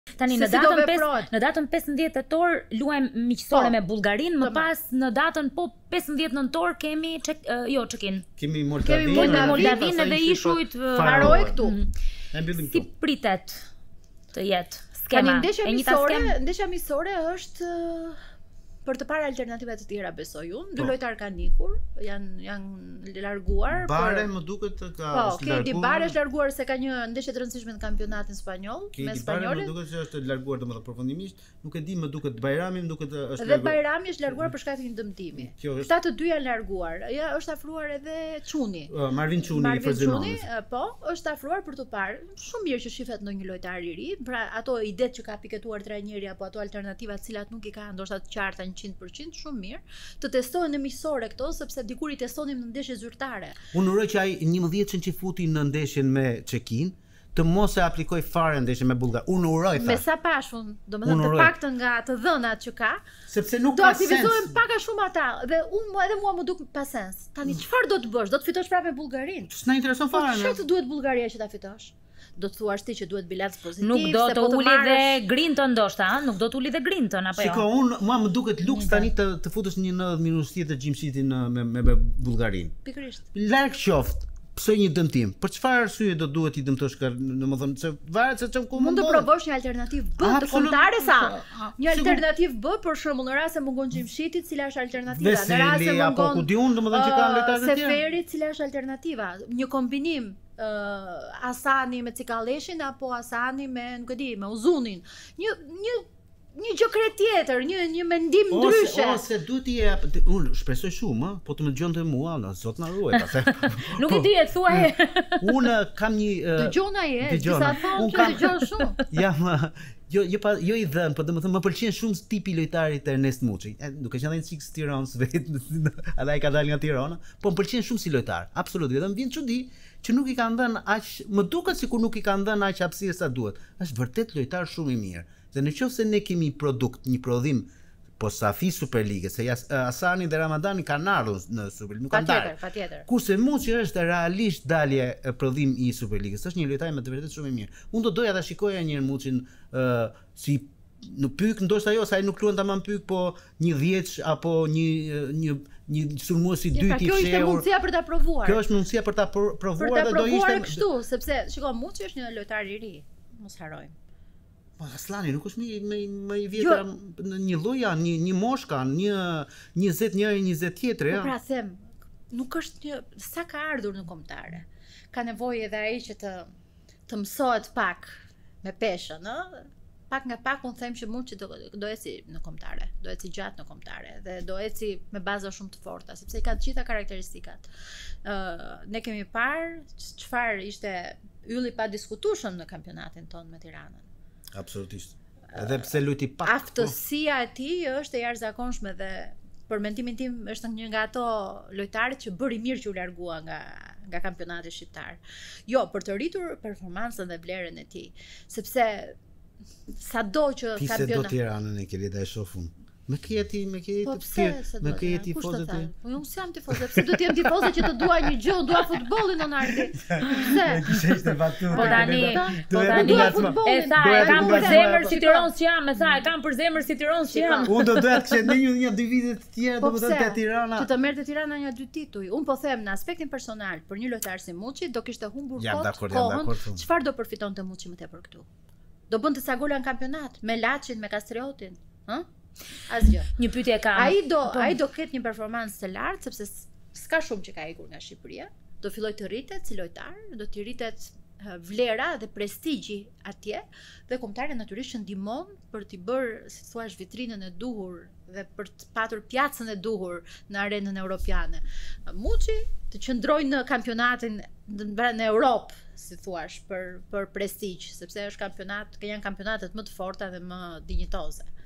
“Shuhet” sporti/ Gazetarja Enkeleida Zeko kritikë ndaj institucioneve
𝐀𝐮𝐭𝐨𝐫 : NTV